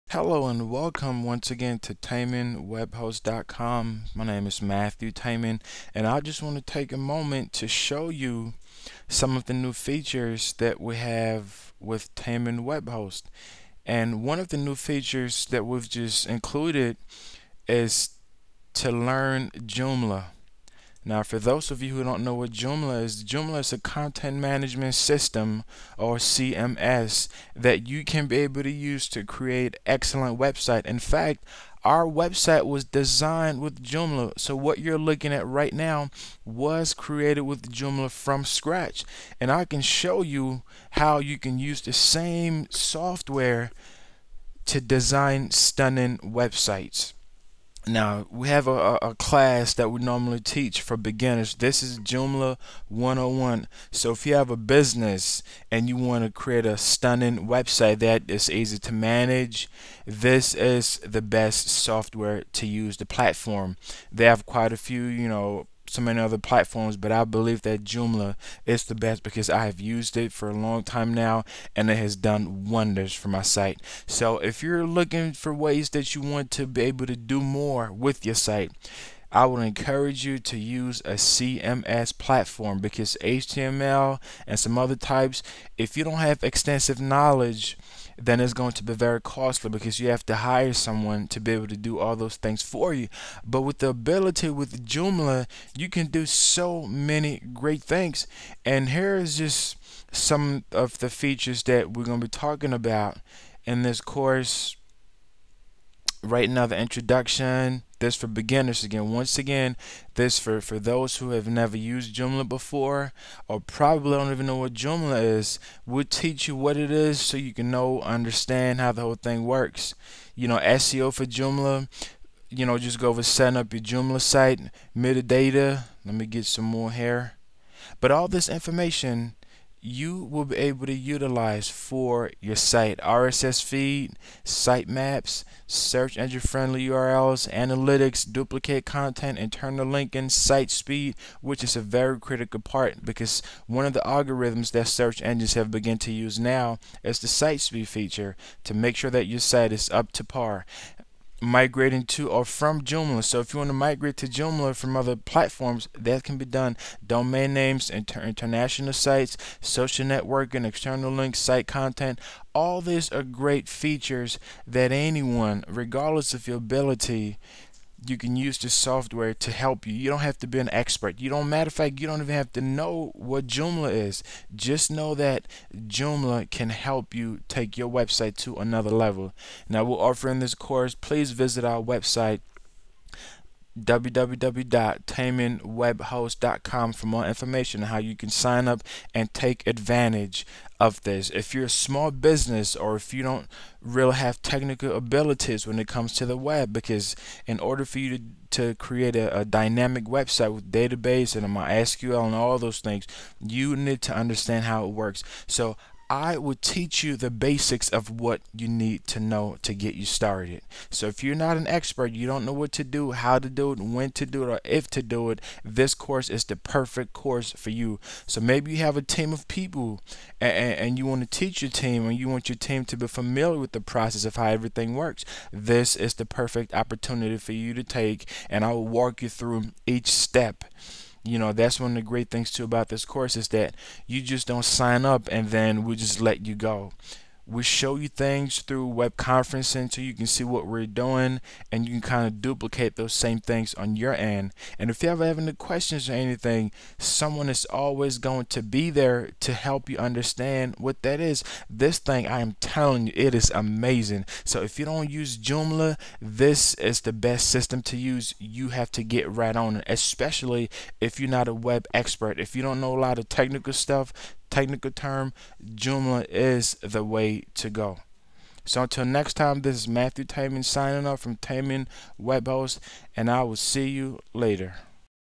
How to Learn Joomla CMS - For Beginners- How To Create A Professional Website
Joomla is the the best CMS (content management system)  to create dynamic websites. The best part is, you don't need to know coding to create your website. You can create a professional website in less than 48hrs with no prior experience.